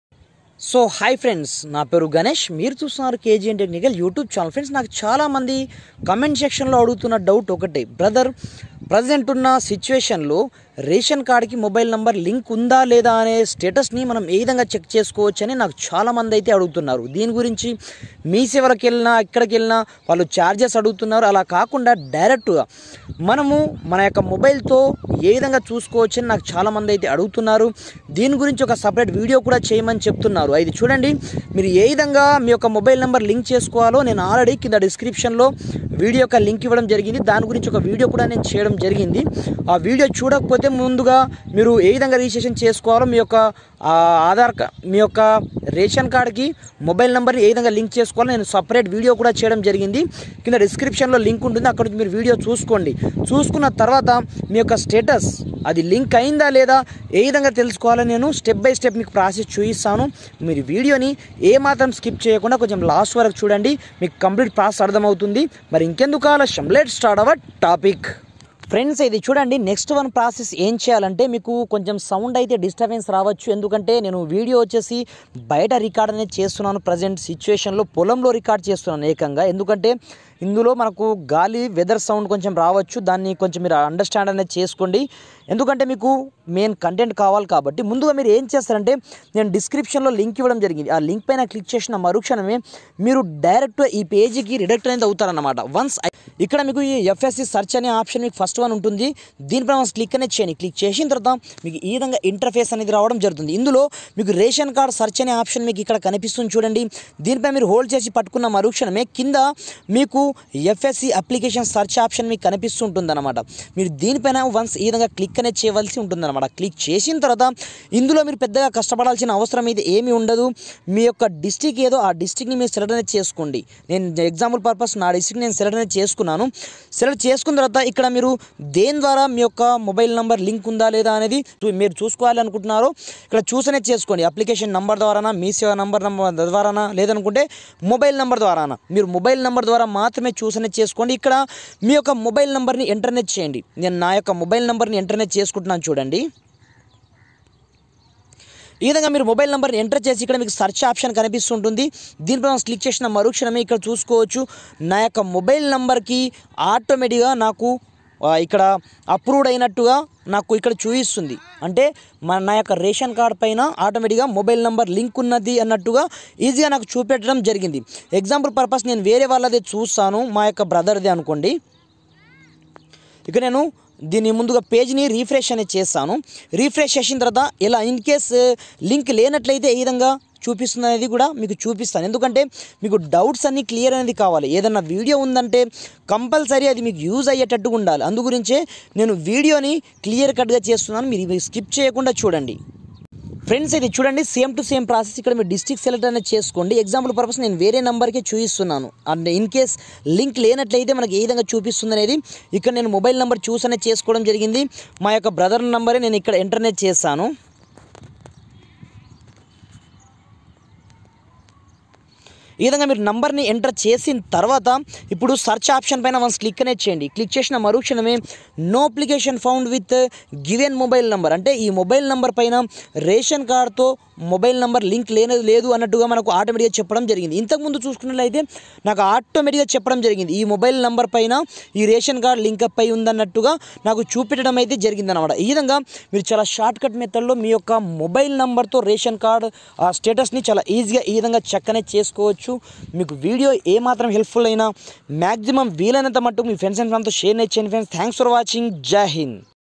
how to check ration card mobile number linking status || రేషన్ కార్డు తో మొబైల్ నెంబర్ లింక్ చెక్ చే
how to check ration card mobile number linking status || రేషన్ కార్డు తో మొబైల్ నెంబర్ లింక్ ఉందా లేదా అనేది చెక్ చేసుకోండి ఇలా